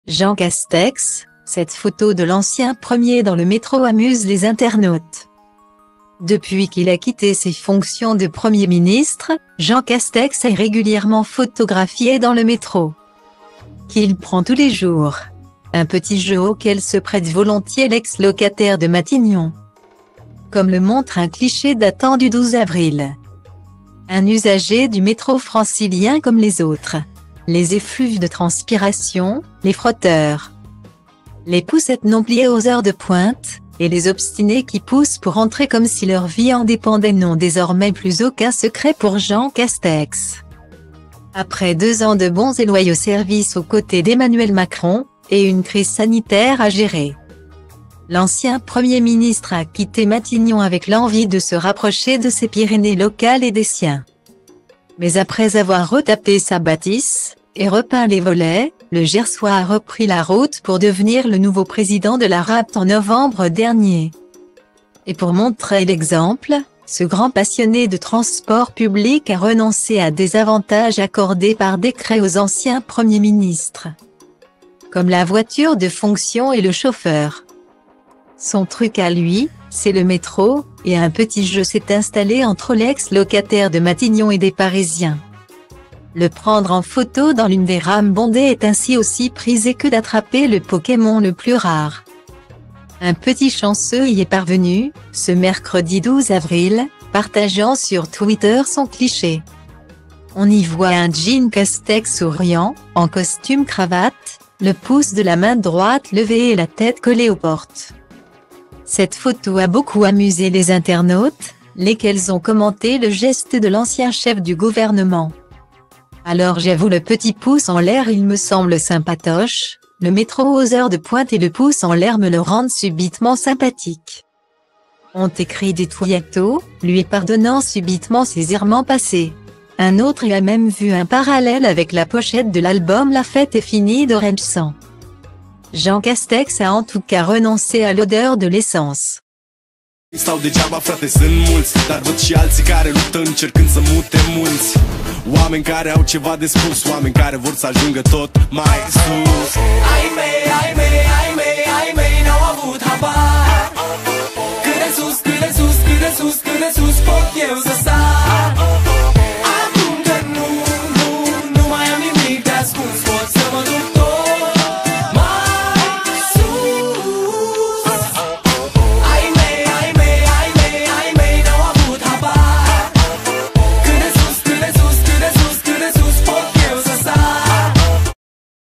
Jean Castex : cette photo de l'ancien Premier dans le métro amuse les internautes
Depuis qu'il a quitté ses fonctions de Premier ministre, Jean Castex est régulièrement photographié dans le métro,qu'il prend tous les jours. Un petit jeu auquel se prête volontiers l'ex-locataire de Matignon,comme le montre un cliché datant du 12 avril.